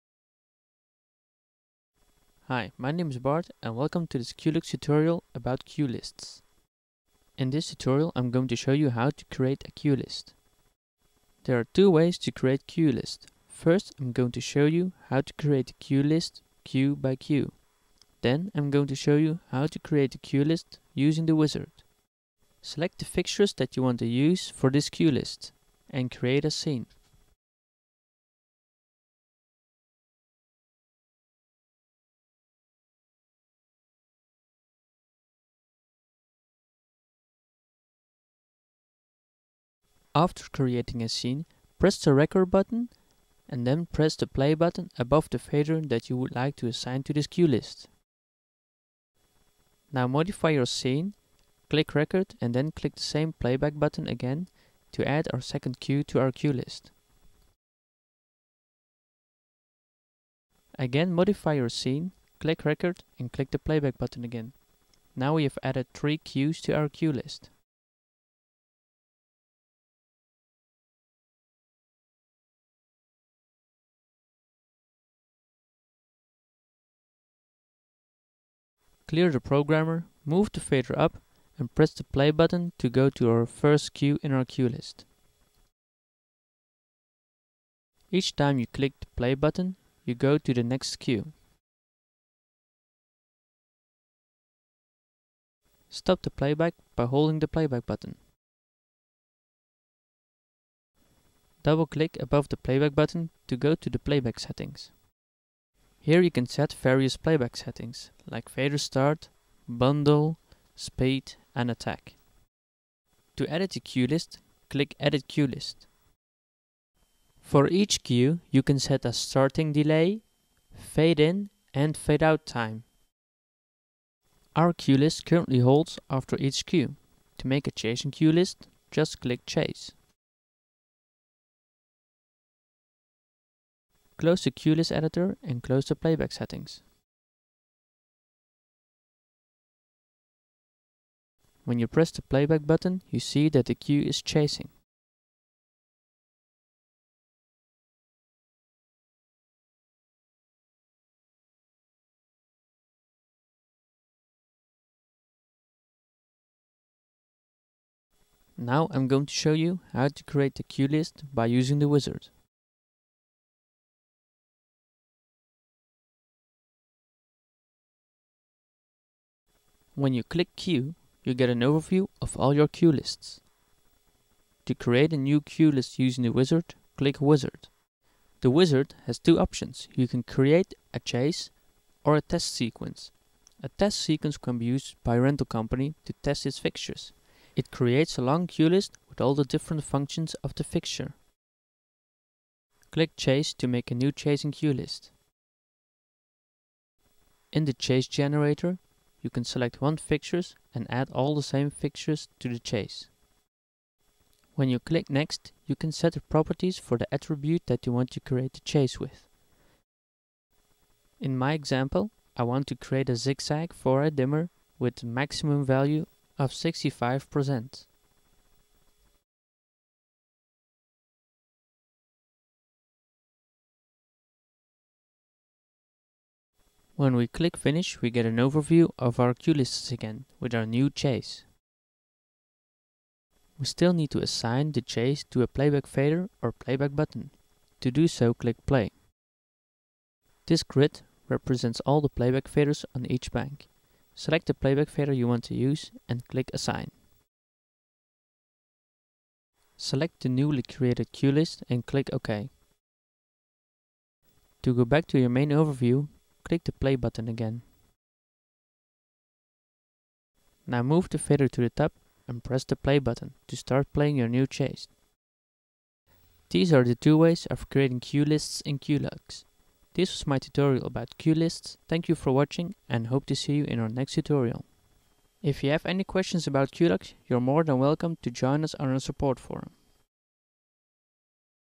Cuelux Tutorial - Creating Cuelists
This tutorial shows the two ways used to create new Cuelists.